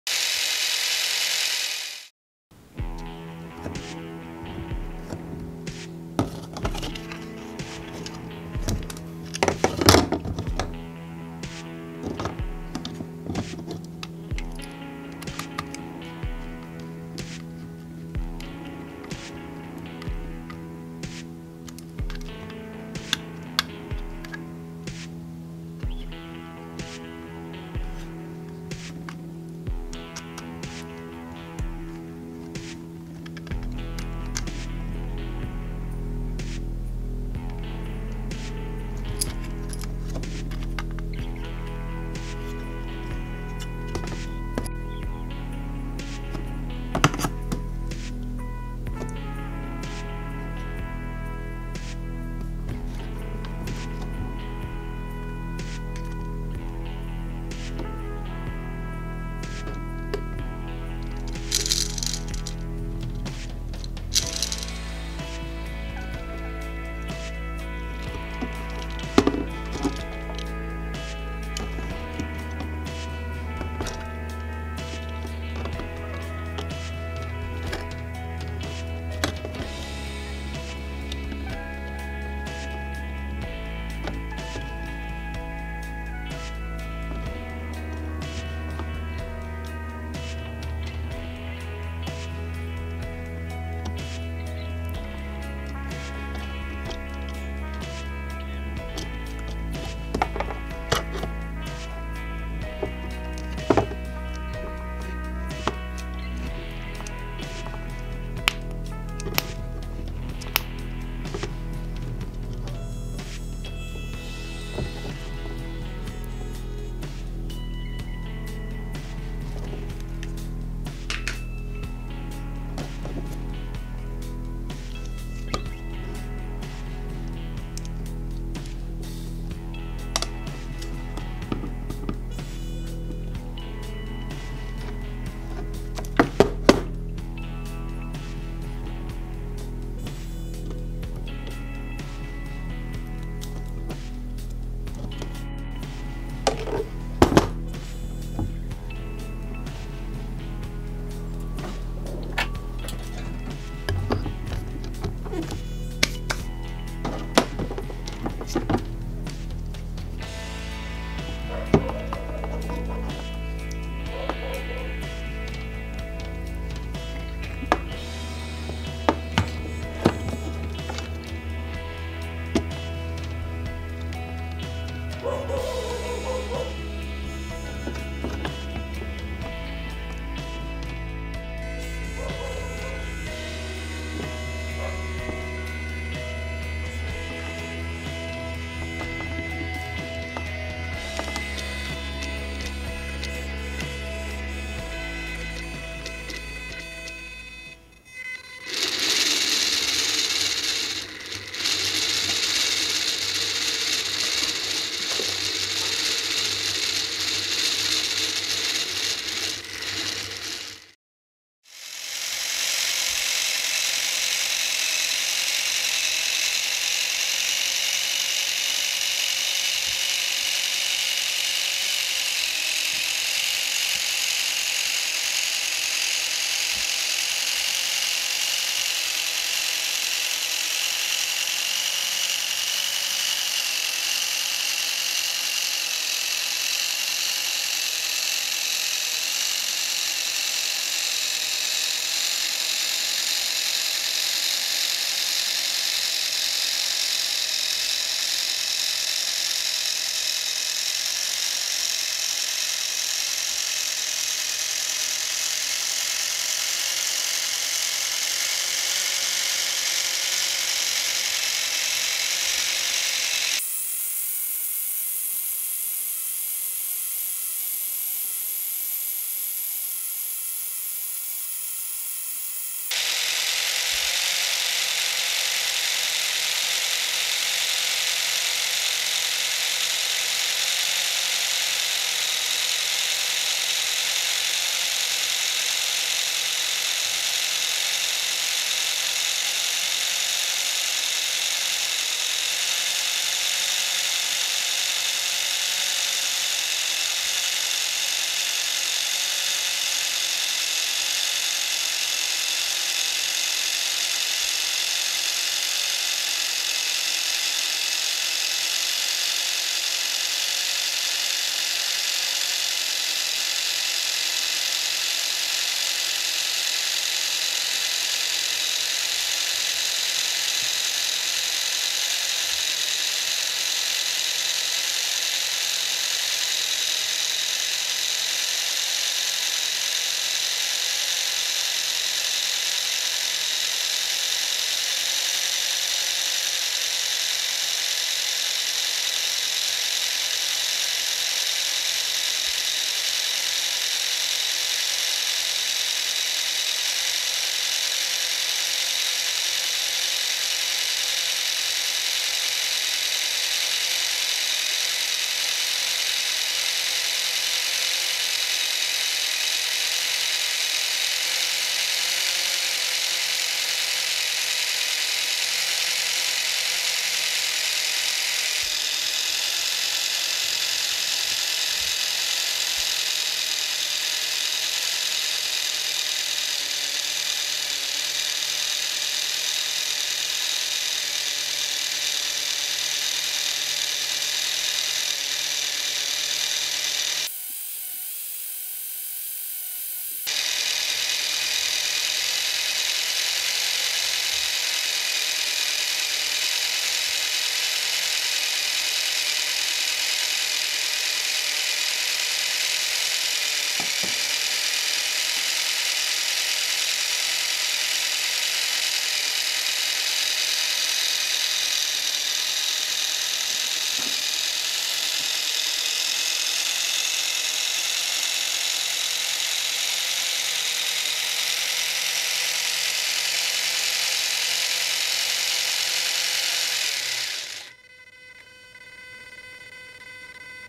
3D Printed - Mercedes rear suspension - Test with gearbox
Hi,
This's a test of my 3D Printed fully feature rear suspension for my Mercedes AMG GT Welly FX 1/24.
This is the first part of a series convert my Mercedes AMG GT Welly FX 1/24 to RC car with all function and parts as real car.